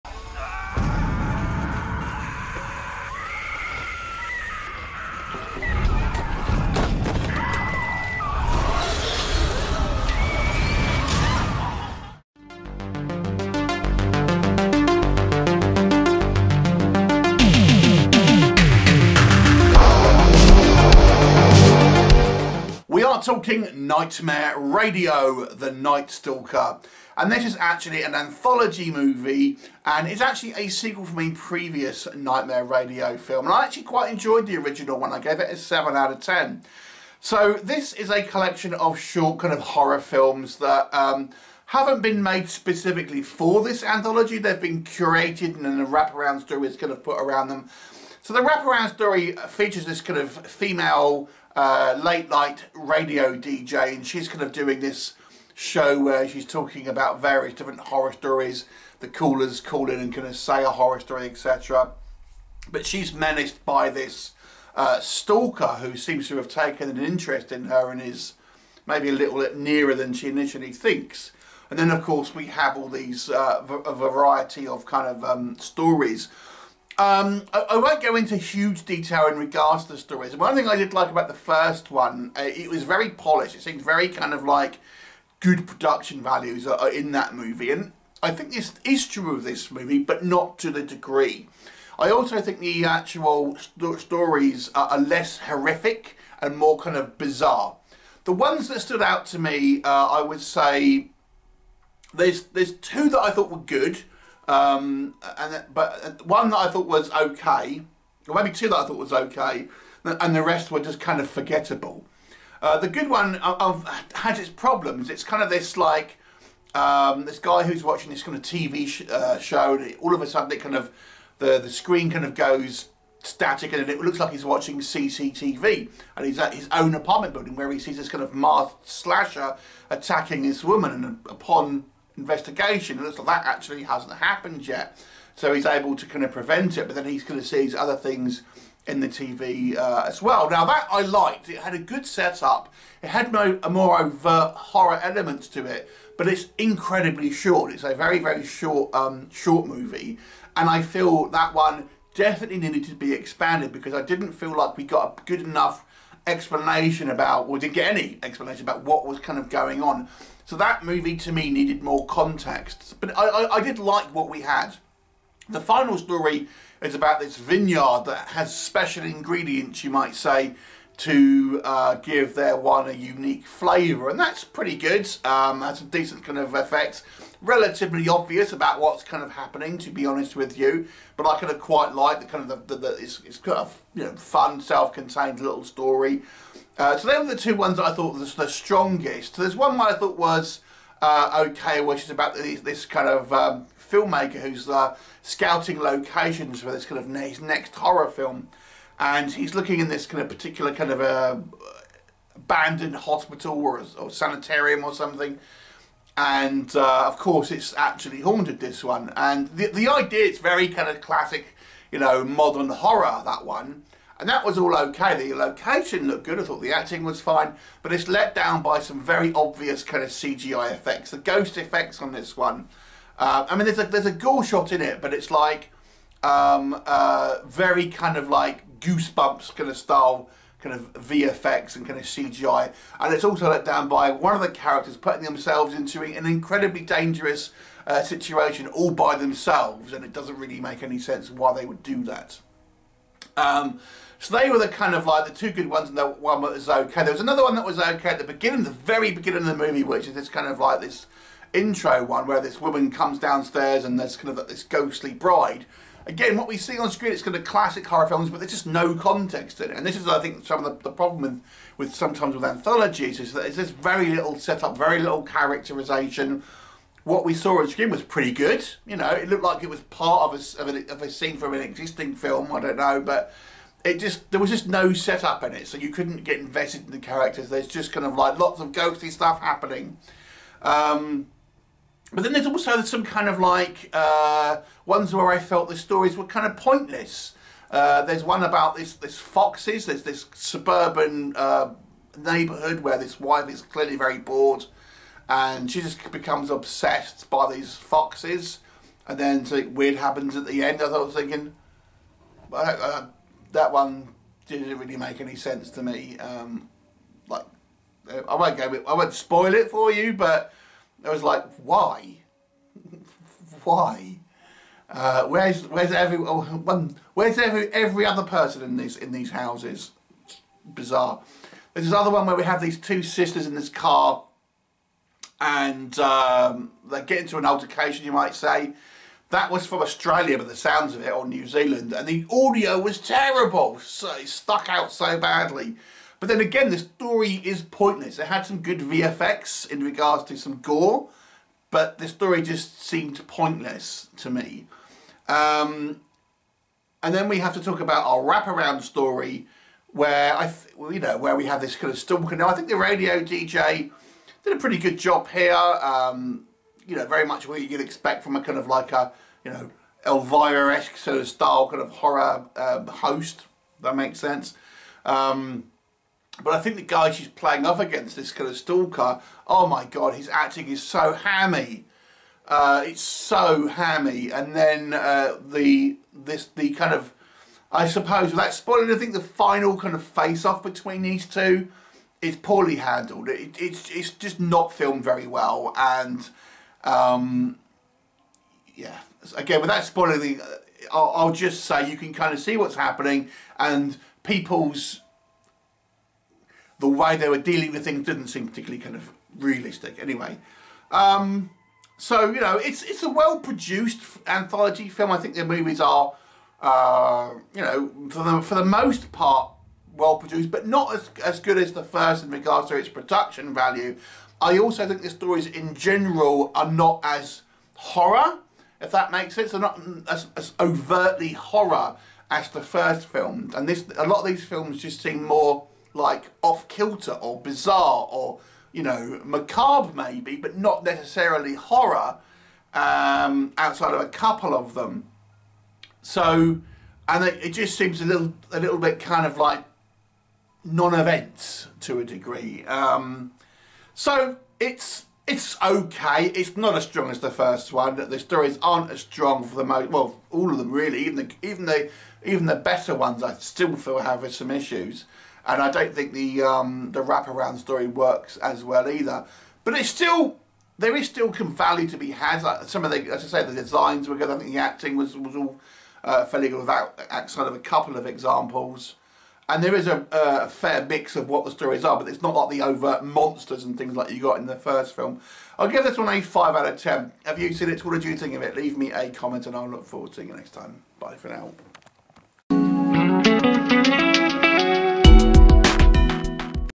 NIGHTMARE RADIO : THE NIGHT STALKER ( 2023 Paula Brasca ) Anthology Horror Movie Review
Candy is a late-night radio DJ with a program where people call to tell her real horror stories. One of her listeners begins to call her insistently. Candy quickly discovers that there is a strange connection between them. Soon she will find out how far an obsessed fan is willing to go.